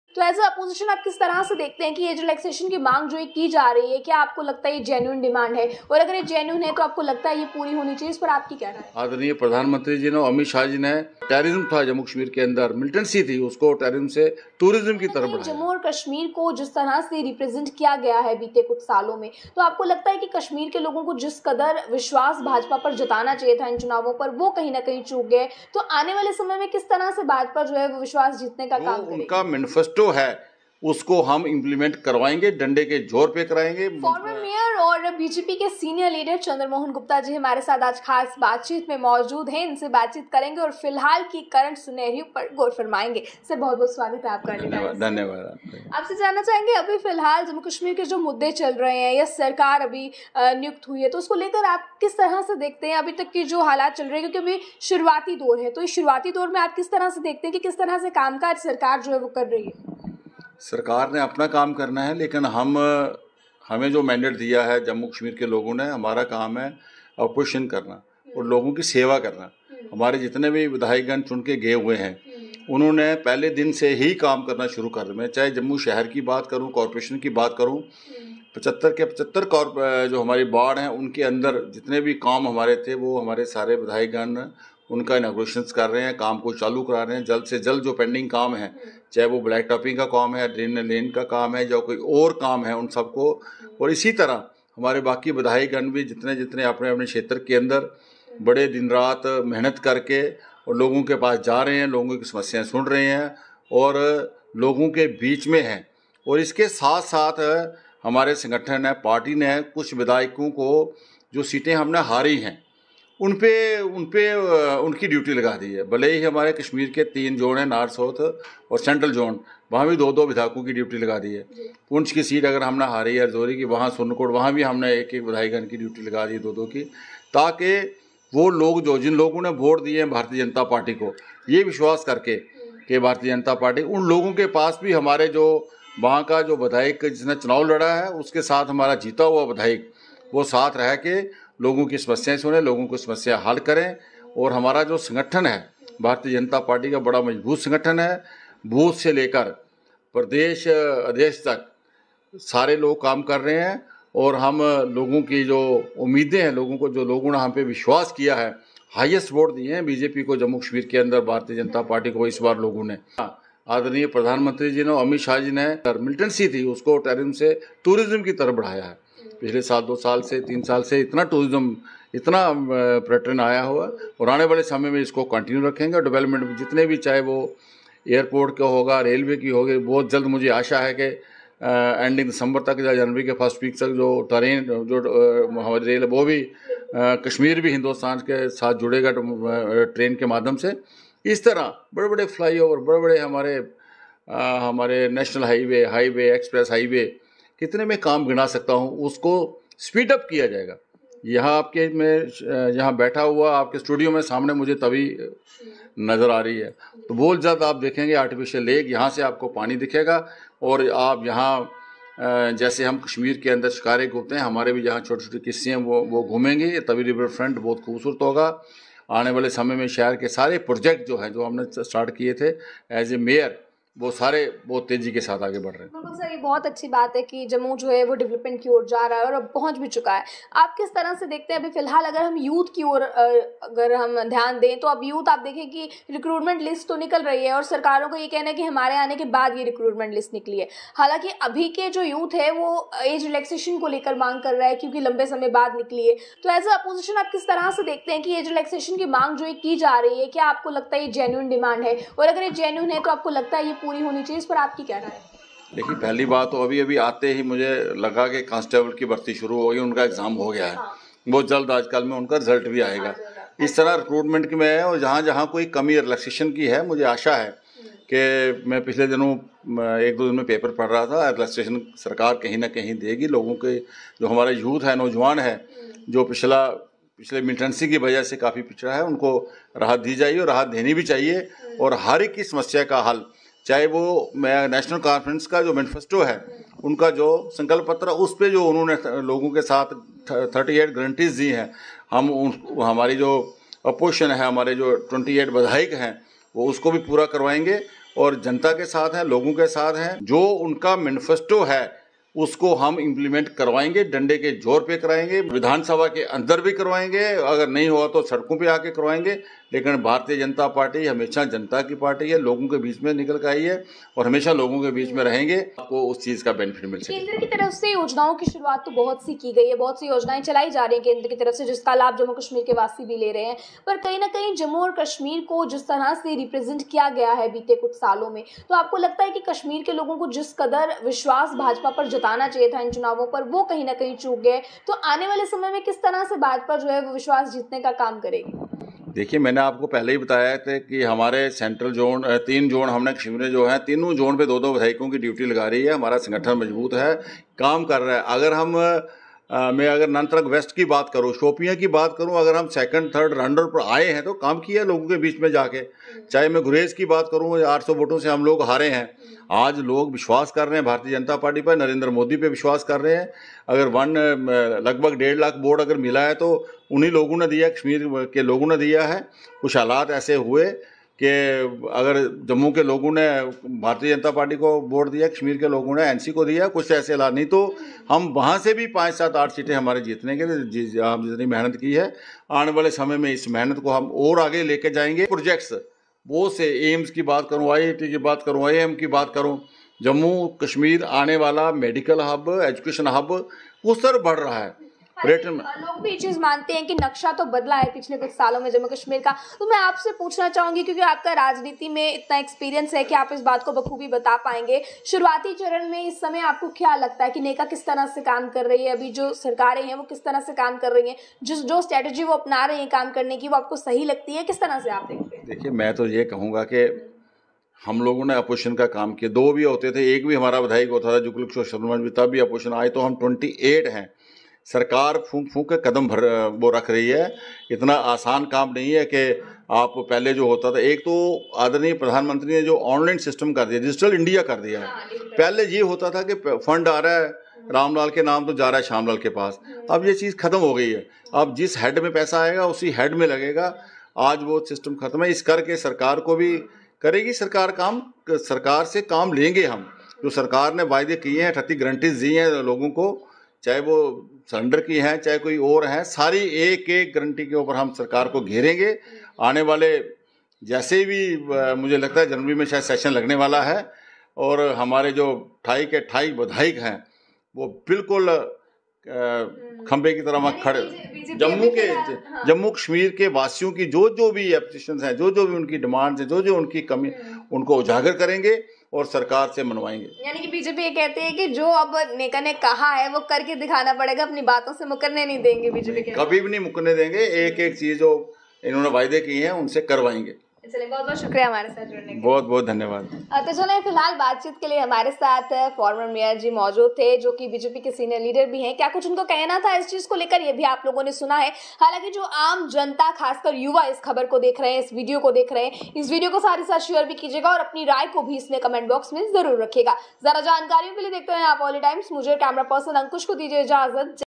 Kya Aspirants ko Age relaxation milni chahiye ?Conversation with Chander Mohan Gupta
Kya Aspirants ko Age relaxation milni chahiye ?
Conversation with Chander Mohan Gupta